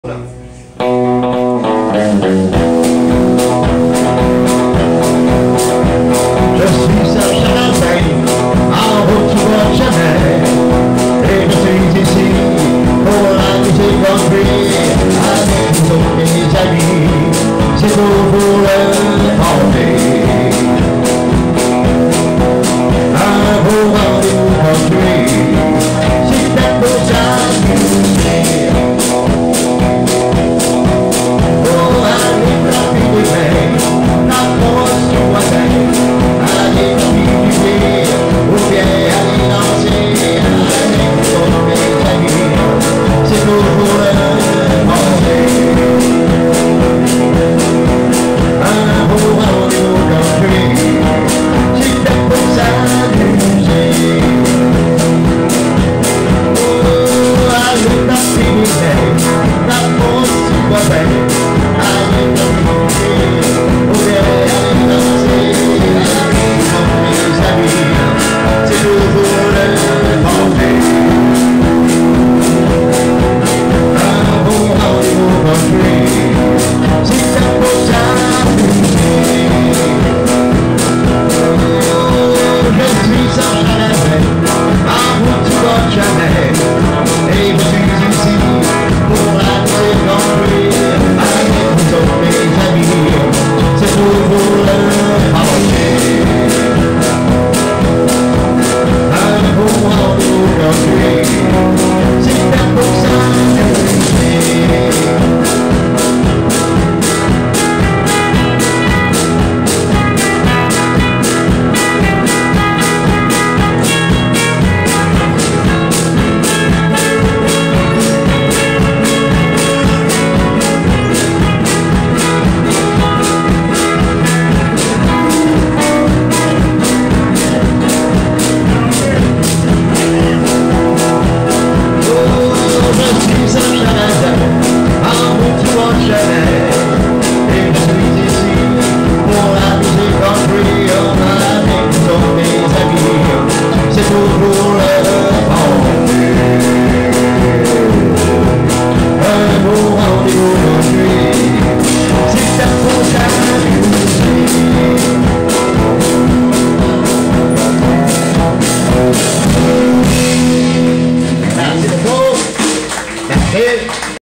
Serge Sarazin - Tulip Valley - 14-09-2019 - Chelsea Qc
Serge Sarazin -- Vocal And Bass -- Yvon Monette    On Lead Guitar -- Anthony Brascoupé On Rhythm Guitar And Mike Éthier On Drums -- Tulip Valley -- Samedi 14 Septembre 2019 -- Chelsea Québec